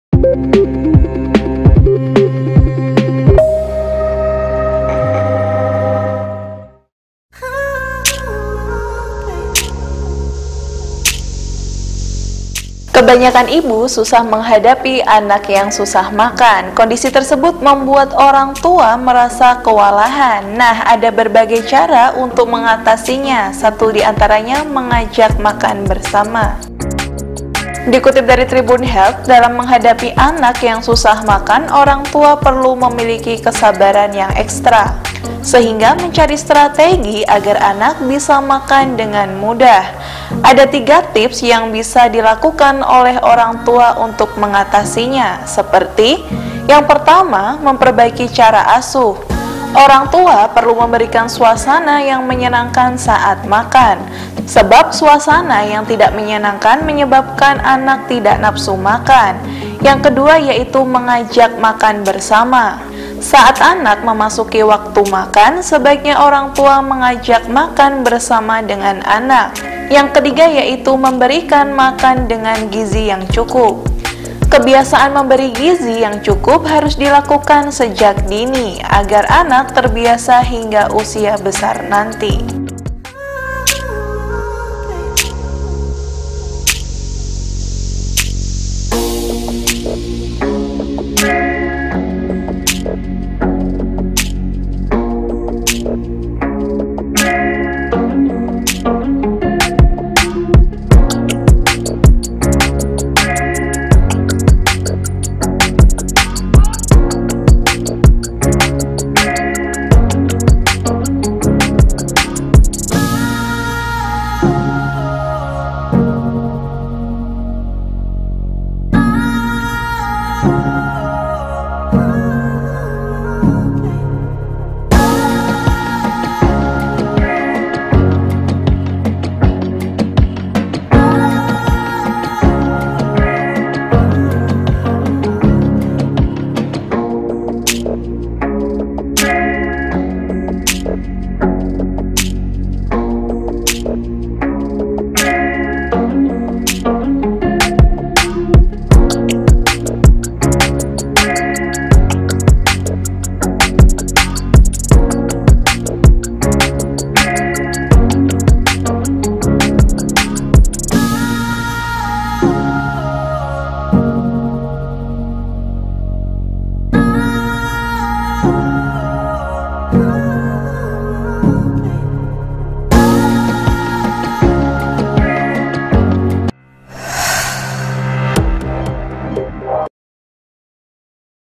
Anak Susah Makan? Ini 3 Cara Mudah Mengatasi Anak Susah Makan dengan Peran Orangtua
Kebanyakan ibu susah menghadapi anak yang susah makan.

Kondisi tersebut membuat orangtua merasa kewalahan.

Ada berbagai cara untuk mengatasinya, satu di antaranya mengajak makan bersama.

Dikutip dari TribunHealth, Dalam menghadapi anak yang susah makan orangtua perlu memiliki kesabaran yang ekstra.

Sehingga mencari strategi agar anak bisa makan dengan mudah.